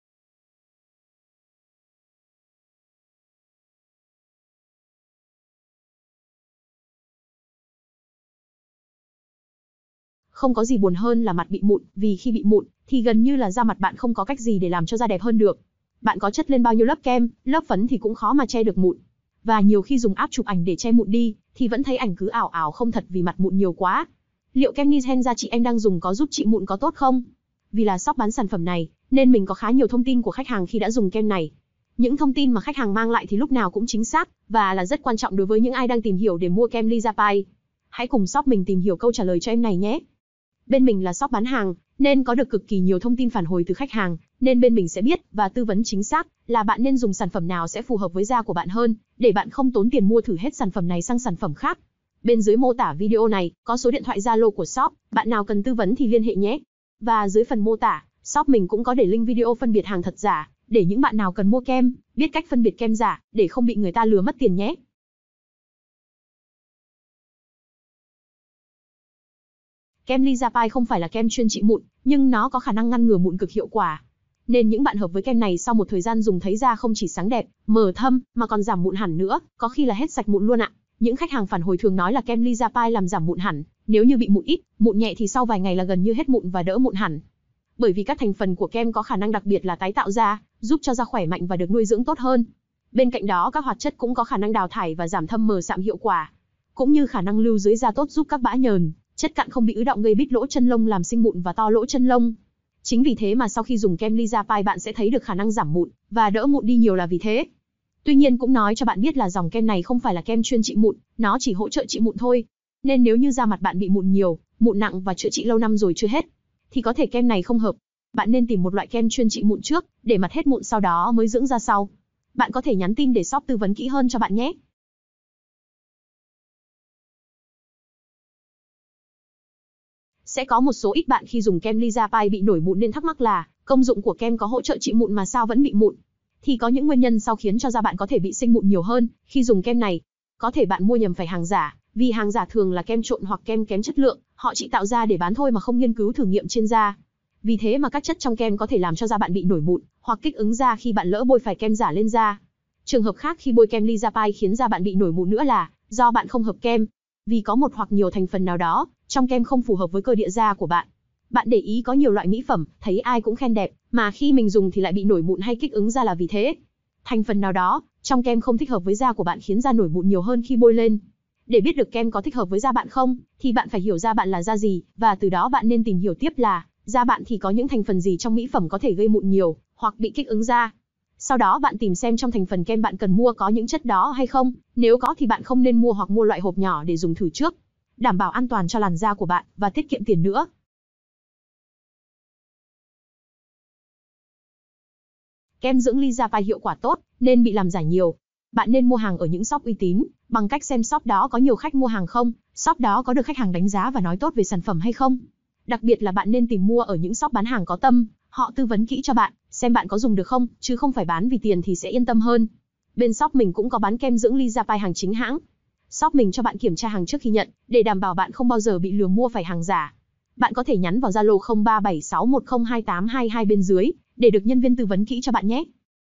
Kem Liyapai Có Trị Mụn Không? Kem Trắng Da Liyapai Có Gây Mụn Không?
Không có gì buồn hơn là mặt bị mụn, vì khi bị mụn thì gần như là da mặt bạn không có cách gì để làm cho da đẹp hơn được. Bạn có chất lên bao nhiêu lớp kem, lớp phấn thì cũng khó mà che được mụn. Và nhiều khi dùng app chụp ảnh để che mụn đi thì vẫn thấy ảnh cứ ảo ảo không thật vì mặt mụn nhiều quá. Liệu Kem nizhenya chị em đang dùng có giúp trị mụn có tốt không? Vì là shop bán sản phẩm này, nên mình có khá nhiều thông tin của khách hàng khi đã dùng kem này. Những thông tin mà khách hàng mang lại thì lúc nào cũng chính xác và là rất quan trọng đối với những ai đang tìm hiểu để mua Kem Liyapai. Hãy cùng shop mình tìm hiểu câu trả lời cho em này nhé.